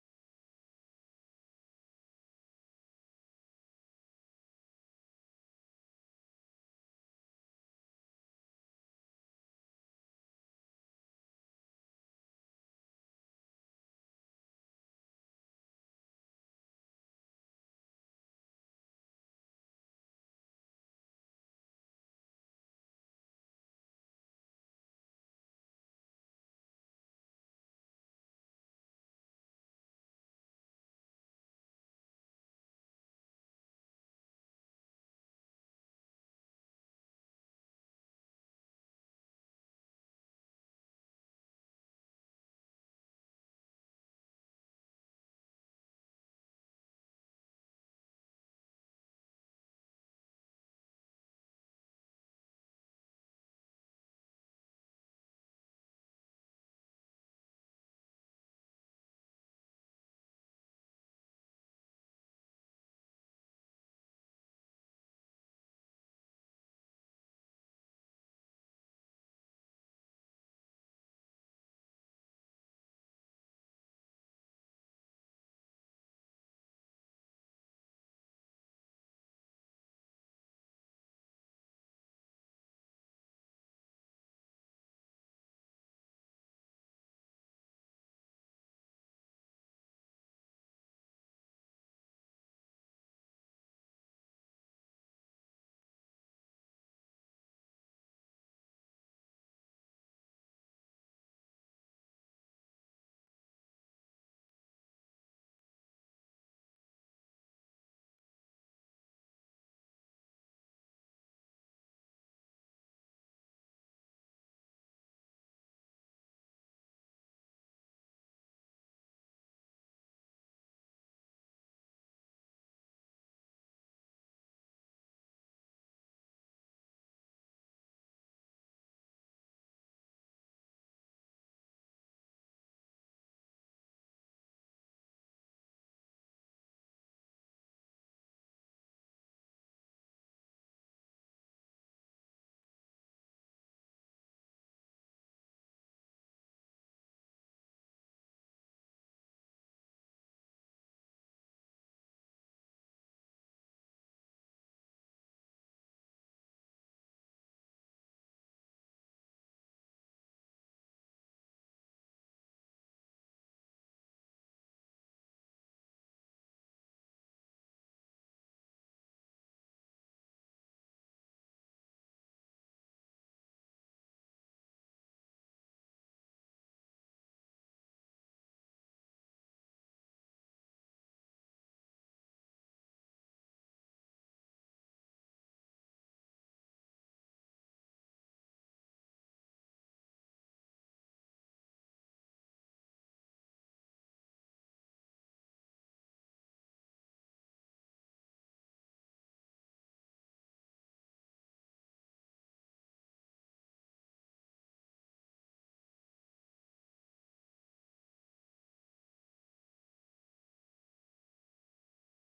Sam- HSE Trillium Show 2014
Sam's rounds at the HSE Trillium Show on Copernicus, 0.90m jumpers